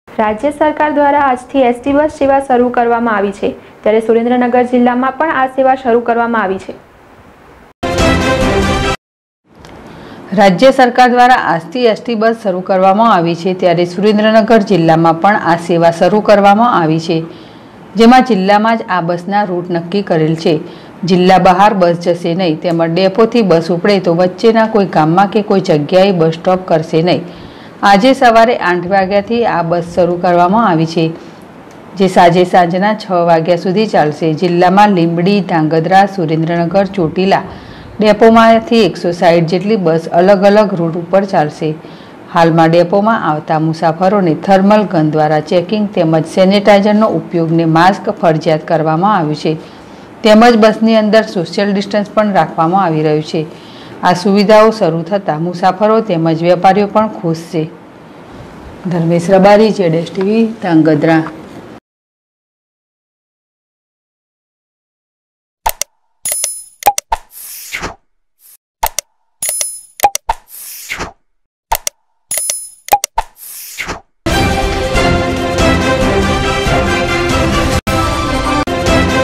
સુરેન્દ્રનગર જિલ્લામાં એસ.ટી. બસ શરૂ રાજ્ય સરકાર દ્વારા આજ થી એસ ટી. બસ શરૂ કરવામાં આવી છે
સુરેન્દ્રનગર જિલ્લામાં એસ.ટી. બસ શરૂ રાજ્ય સરકાર દ્વારા આજ થી એસ ટી.